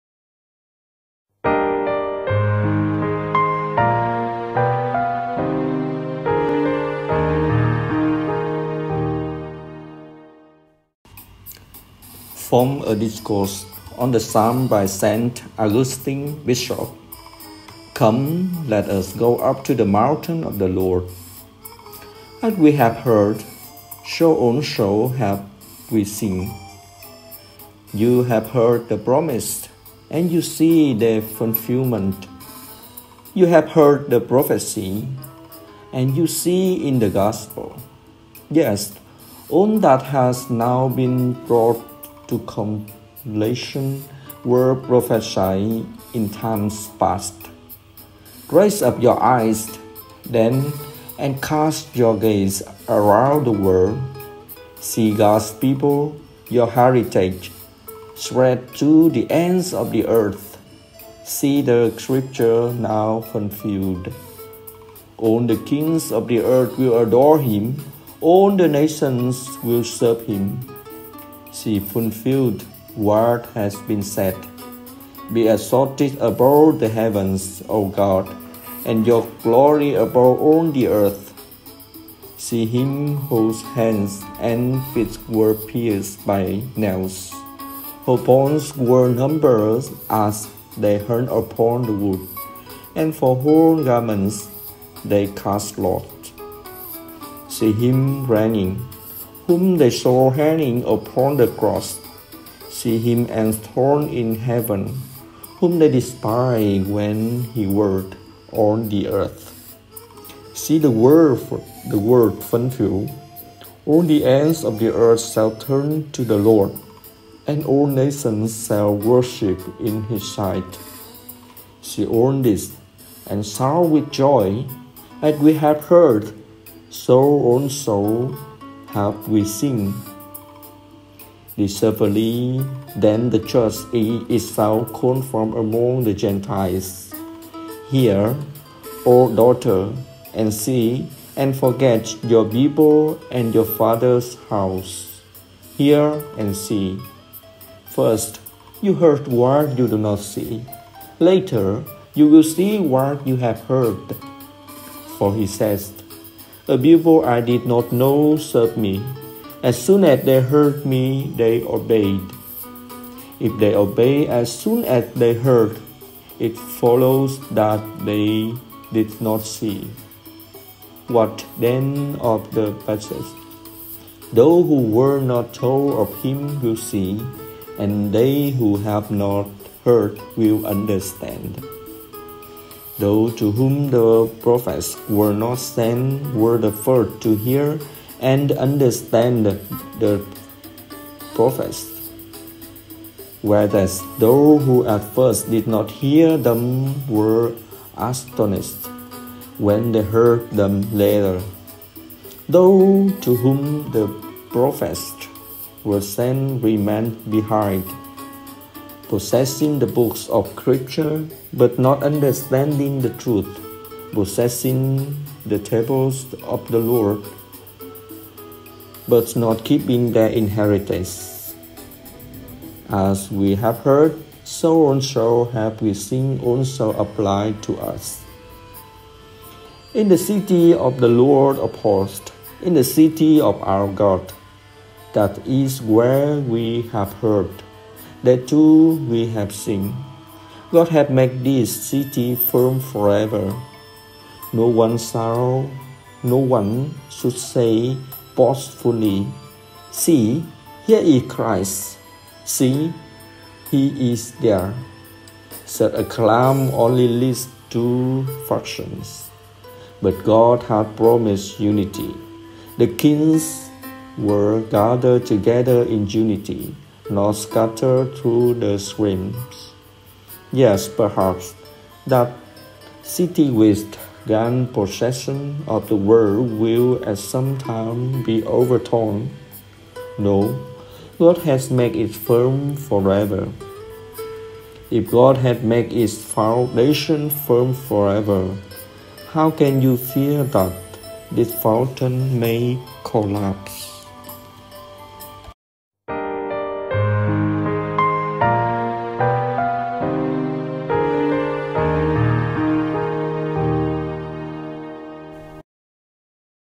JDTCP. Augustine. Come, let us go up to the mountain of the Lord
From a discourse on the psalms by Saint Augustine, bishop
(Ps. 47, 7: CCL 38, 543-545)

Come, let us go up to the mountain of the Lord

As we have heard, so also have we seen. You have heard the promises, and you see their fulfillment; you have heard in prophecy, and you see in the Gospel. Yes, all that has now been brought to completion was prophesied in times past. Raise up your eyes, then, and cast your gaze around the world. See God’s people, your heritage, spread to the ends of the earth. See the Scripture now fulfilled: All the kings of the earth will adore him, all the nations will serve him. See fulfilled what has been said: Be exalted above the heavens, O God, and your glory above all the earth. See him whose hands and feet were pierced by nails, whose bones were numbered as they hung upon the wood, and for whose garments they cast lots. See him reigning, whom they saw hanging upon the cross; see him enthroned in heaven, whom they despised when he walked on the earth. See the word fulfilled: All the ends of the earth shall turn to the Lord, and all nations shall worship in his sight. See all this and shout with joy: As we have heard, so also have we seen.

Deservedly then the Church is itself called from among the Gentiles: Hear, O daughter, and see, and forget your people and your father’s house. Hear and see. First you hear what you do not see; later you will see what you have heard. For he says: A people I did not know served me, as soon as they heard me they obeyed. If they obeyed as soon as they heard, it follows that they did not see. What then of the passage: Those who were not told of him will see, and they who have not heard will understand? Those to whom the prophets were not sent were the first to hear and understand the prophets, whereas those who at first did not hear them were astonished when they heard them later. Those to whom the prophets were sent remained behind, possessing the books of Scripture but not understanding the truth, possessing the tables of the law but not keeping their inheritance. As we have heard, so also have we seen also applies to us.

In the city of the Lord of hosts, in the city of our God, that is where we have heard; there too we have seen. God has made this city firm for ever. No one should say boastfully: See, here is Christ; see, he is there. Such a claim only leads to factions. But God has promised unity. The kings were gathered together in unity, not scattered through schisms. Yet perhaps that city which had gained possession of the world will at some time be overthrown? No, God has made it firm for ever. If God has made its foundation firm for ever, how can you fear that this foundation may collapse?